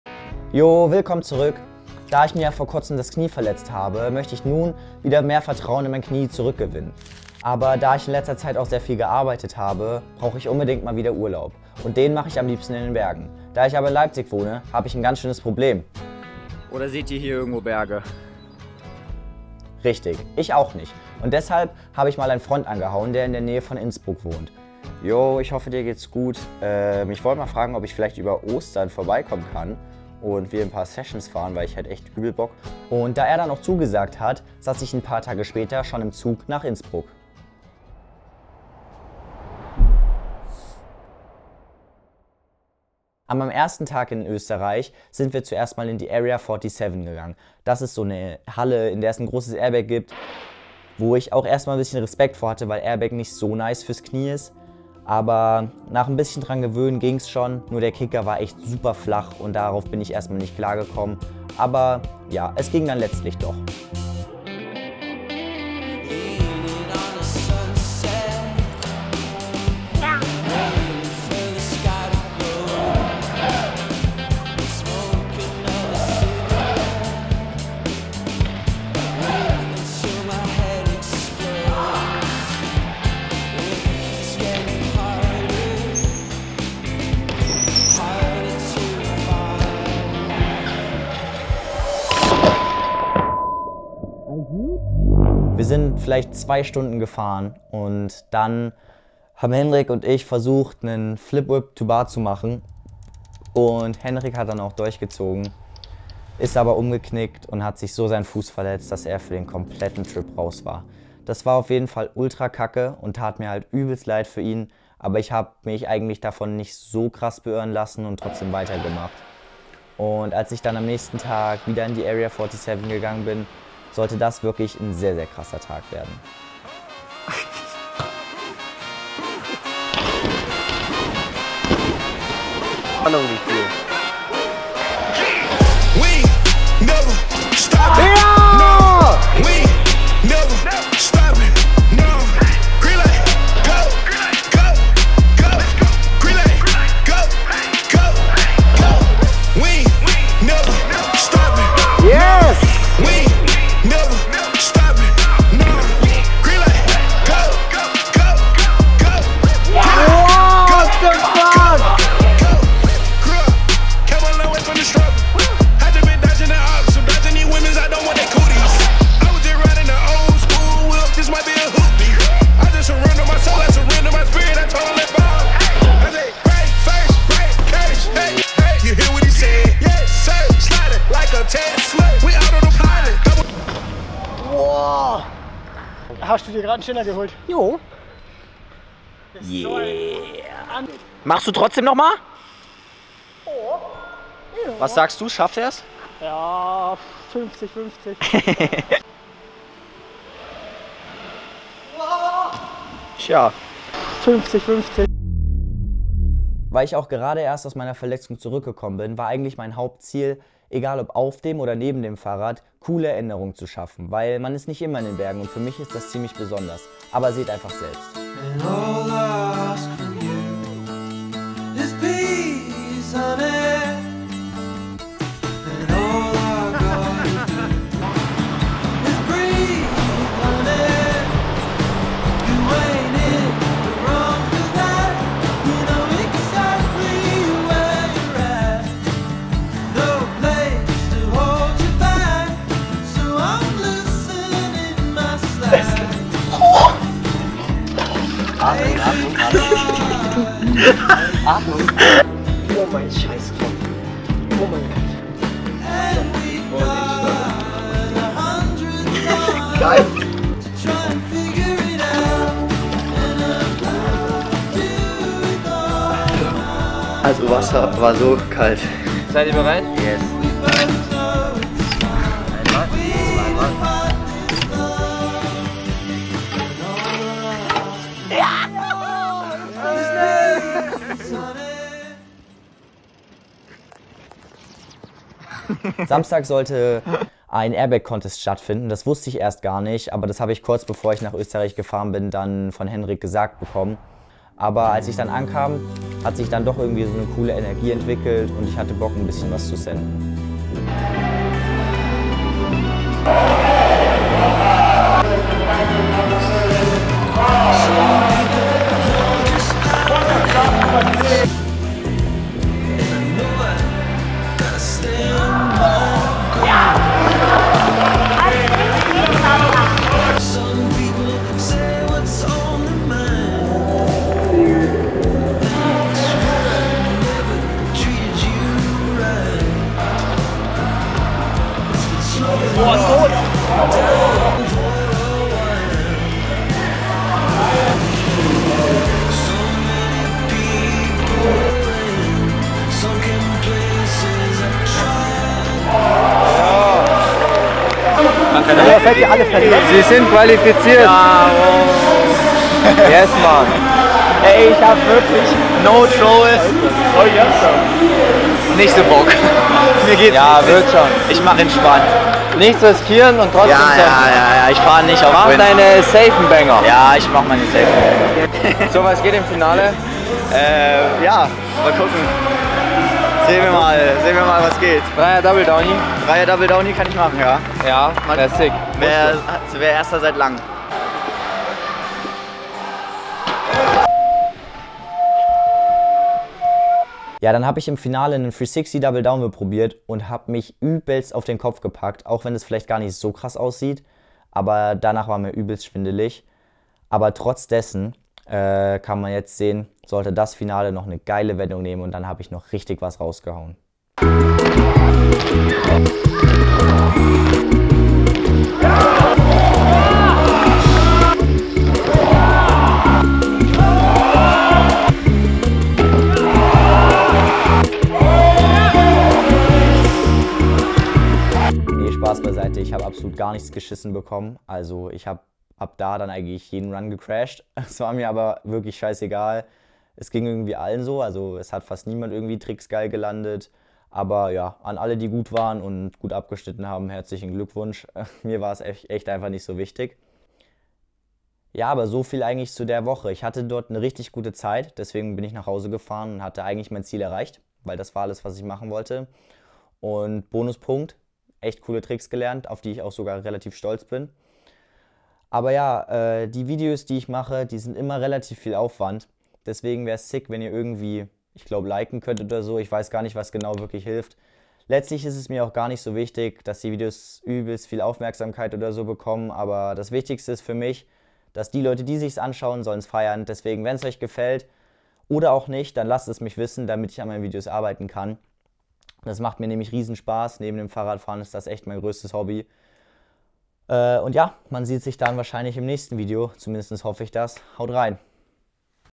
Bester Indoorbikepark Österreich / Full Sends
Erlebe meine besten Sends im Indoorbikepark Area 47 in Österreich! Einen unvergesslichen Trip durch Österreich voller Adrenalin und Erinnerungen.